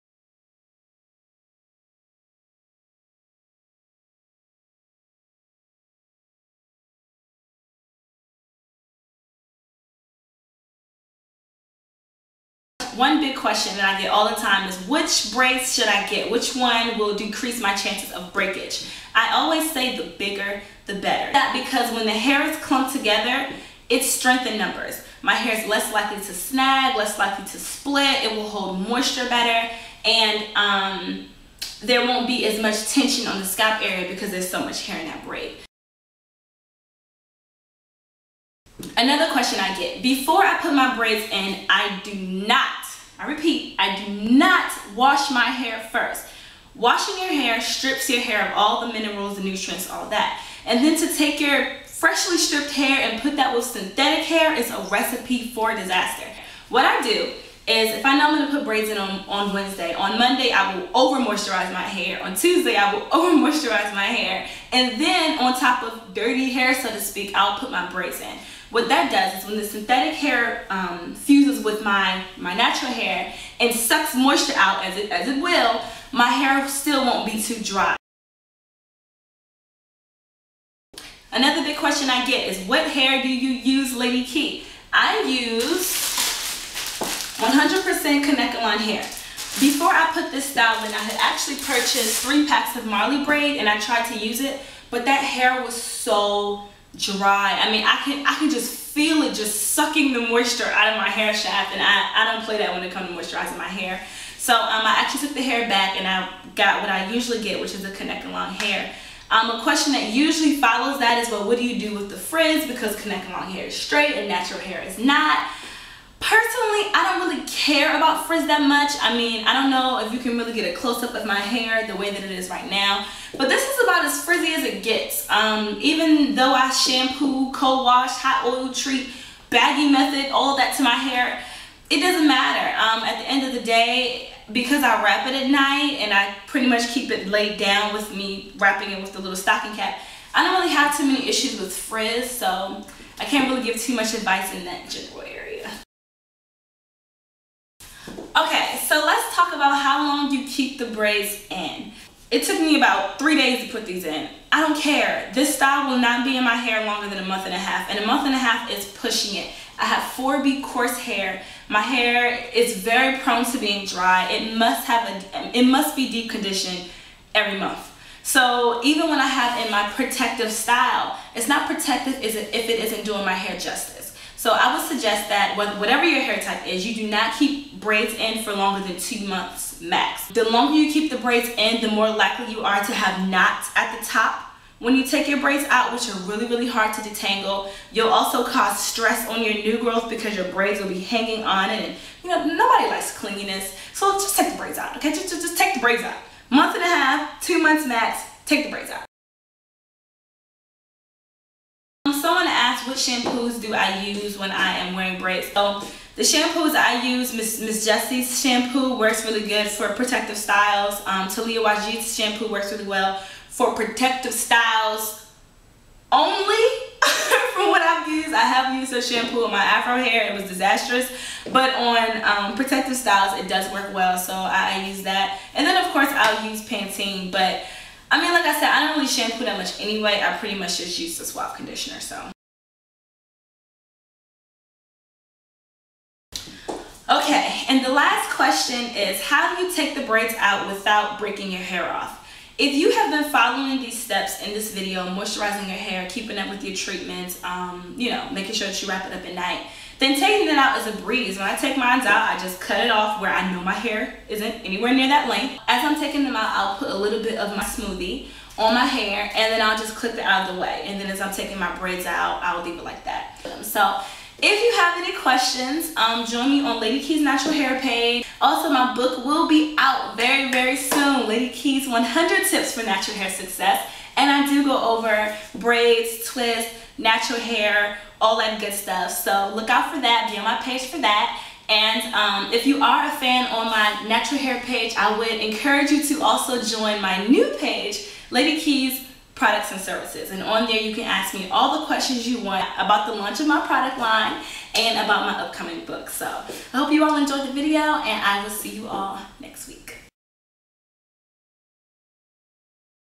How to care for Natural Hair in Braids: Part 2
This video goes over the frequently asked questions from "Lady Key's Natural Hair Page" on how to properly care for your natural hair while it is protected under braids.